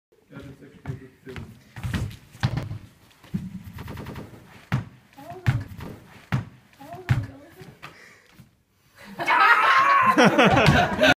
Aerial Practice session: K-Flip Roman & Jojanneke
After lots of hard practice and figuring out the move, we're finally getting there .. in HD + slow motion this time.